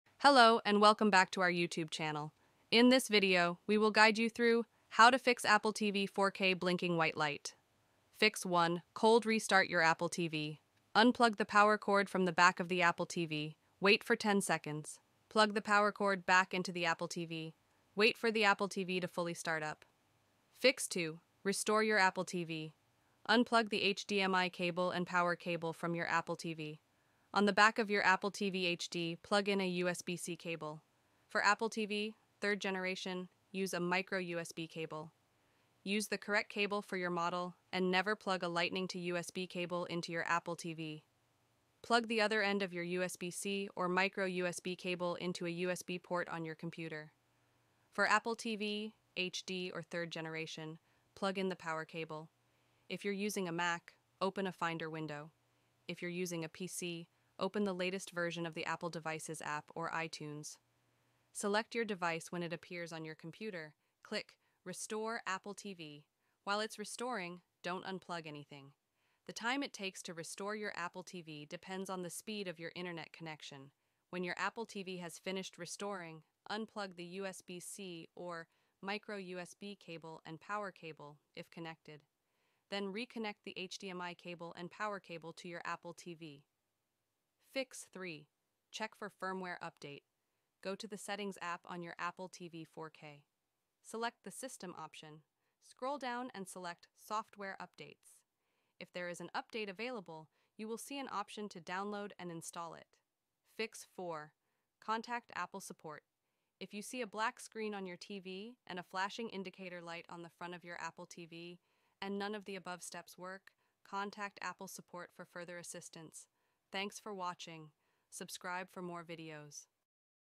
How To Fix Apple TV 4k Blinking White Light
In this video, we will guide you through the steps to fix the common issue of a blinking white light on your Apple TV 4K. Follow along as we troubleshoot and resolve this problem to get your device back up and running smoothly.

00:08 Fix 1: Cold Restart Your Apple TV
00:21 Fix 2: Restore Your Apple TV
01:29 Fix 3: Check For Firmware Update
01:45 Fix 4: Contact Apple support